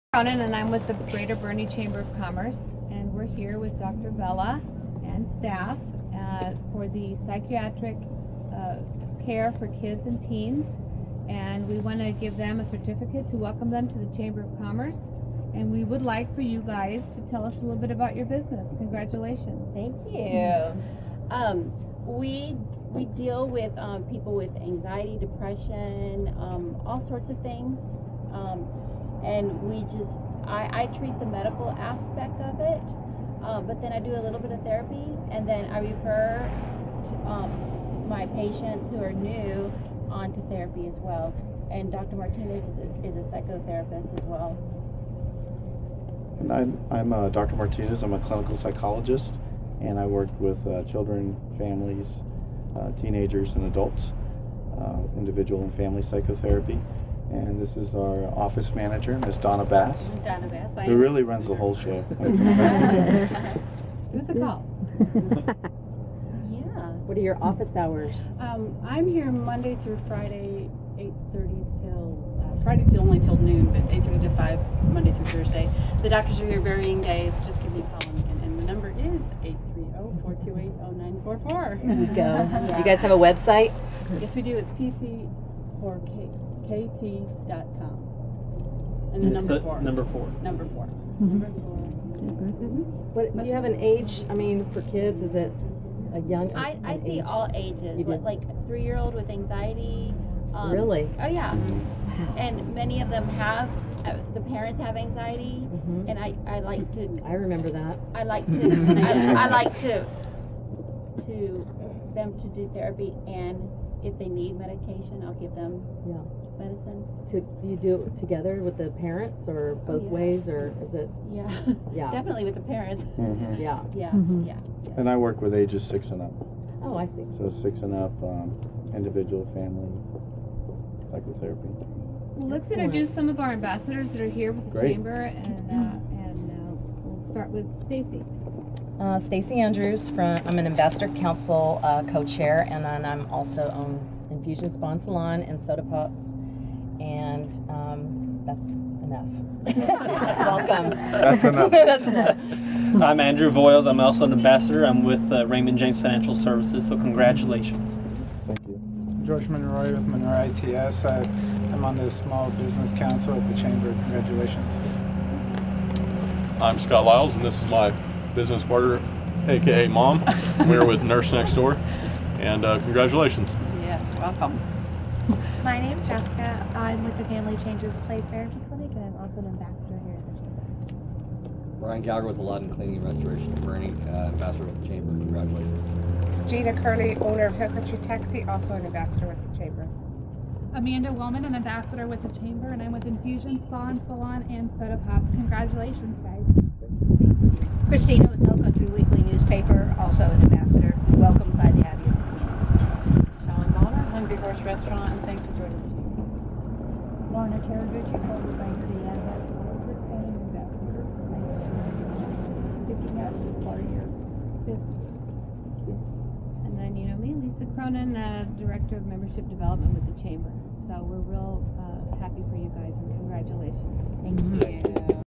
Psychiatric Care for Kids and Teens
Vanessa Vela, M.D. of Psychiatric Care for Kids and Teens, is strongly committed to working with other health professionals in the management and treatment of patients with regard to family dynamics, personal growth, peer relations, and at school or in the workplace. For the last five years, she has served Bexar County. In this time, she has seen a wide spectrum of mental illness from traumatic abuse to severe mental illness. With this professional experience teamed with her personal insights and strong educational foundation, she set forth to practice psychiatry in her home town, Boerne, Texas. Psychiatric Care for Kids and Teens is located at 109 Falls Court, Ste. 600 in Boerne.